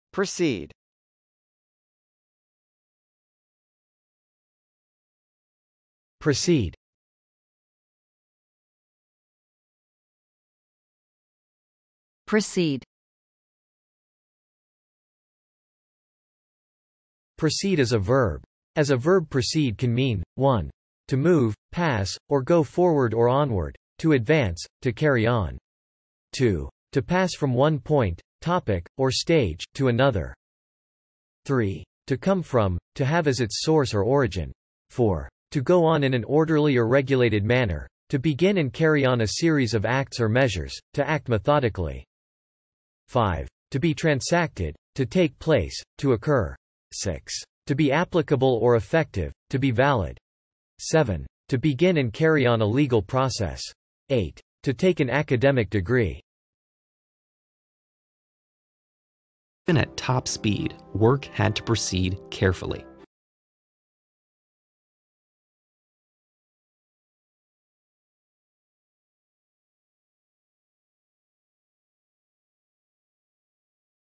PROCEED - Meaning and Pronunciation
How to pronounce proceed?

This video provides examples of American English pronunciations of proceed by male and female speakers.
In addition, it explains the meaning of proceed through a dictionary definition and several visual examples.

IPA Transcription of proceed is /prəsˈid/.

Definition of proceed according to Wiktionary:
proceed is a verb

As a verb proceed can mean:
1. To move, pass, or go forward or onward; to advance; to carry on.
2. To pass from one point, topic, or stage, to another.
3. To come from; to have as its source or origin.
4. To go on in an orderly or regulated manner; to begin and carry on a series of acts or measures; to act methodically.
5. To be transacted; to take place; to occur.
6. To be applicable or effective; to be valid.
7. To begin and carry on a legal process.
8. To take an academic degree.

If you'd like to test your pronunciation of proceed, check out Accent Hero's real-time pronunciation feedback tool: